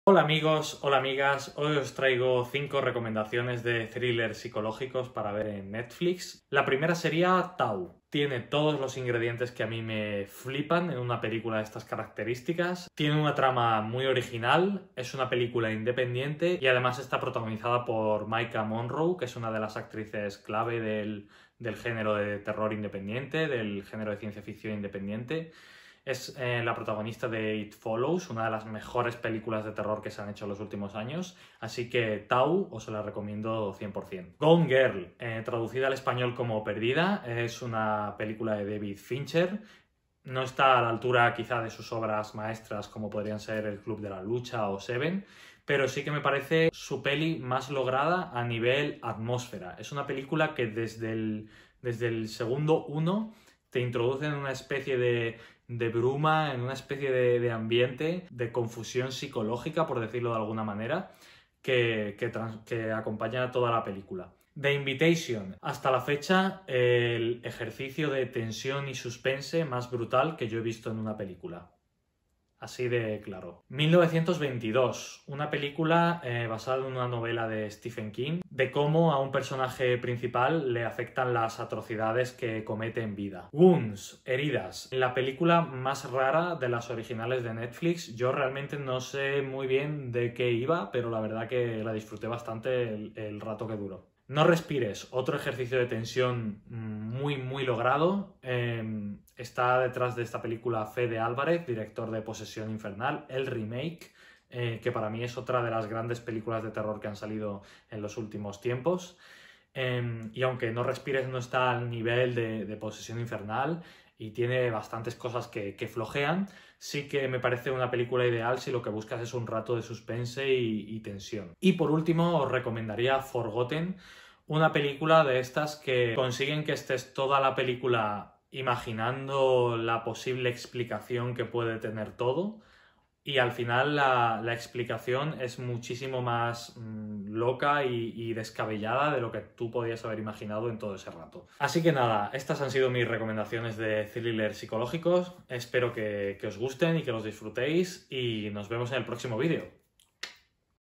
TOP: MEJORES THRILLERS PSICOLÓGICOS EN NETFLIX 2020 PARA LA CUARENTENA | Recomendaciones de pelis
¡Hola amigos y amigas! Os traigo seis recomendaciones de thrillers psicológicos para ver en Netflix.